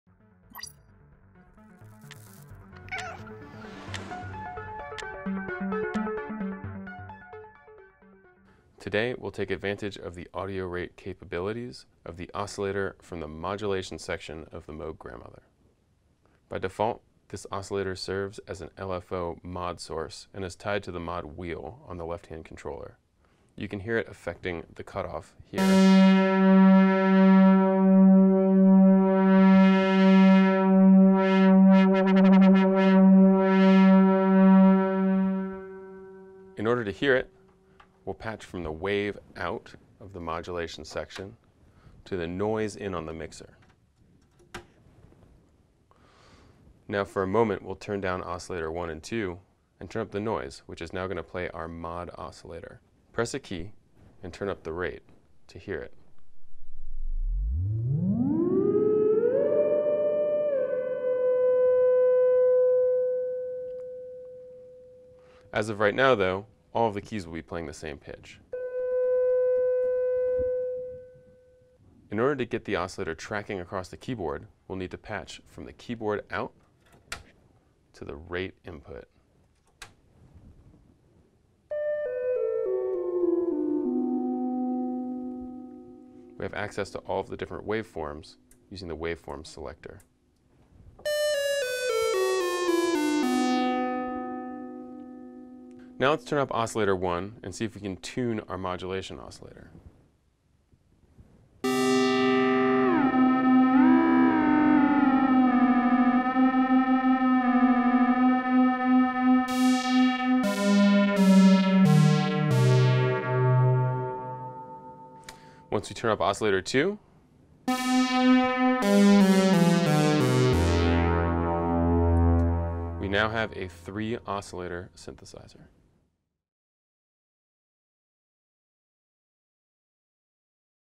Grandmother | Patching the LFO as a Third Oscillator
Using Grandmother’s front panel patch points, you can easily reconfigure the instrument’s on-board analog LFO to act as a third oscillator.

To get notified when new tutorials in the Moog Demo Library are released sign up for our newsletter